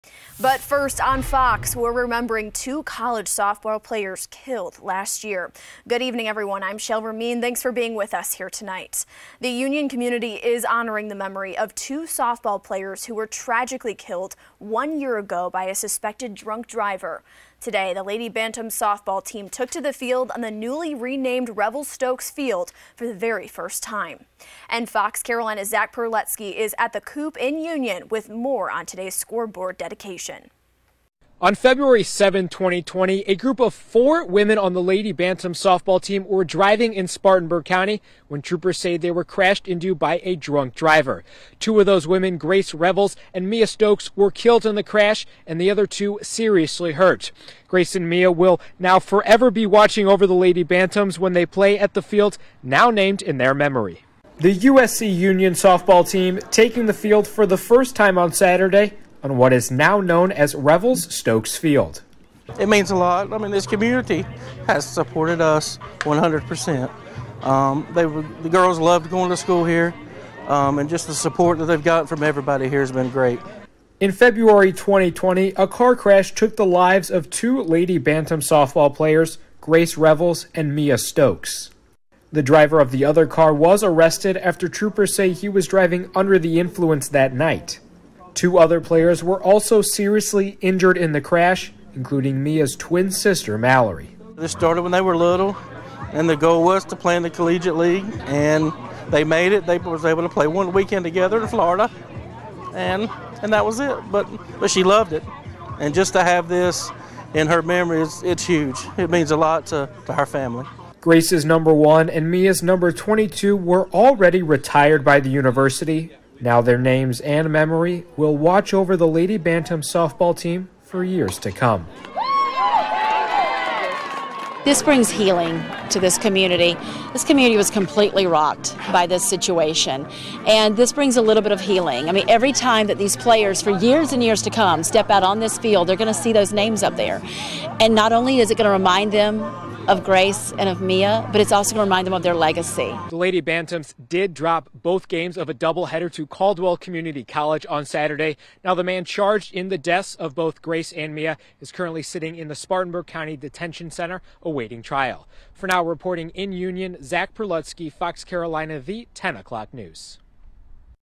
USC Union softball renames scoreboard in honor of two former players who died in fatal wreck
The wreck occurred in February of 2020.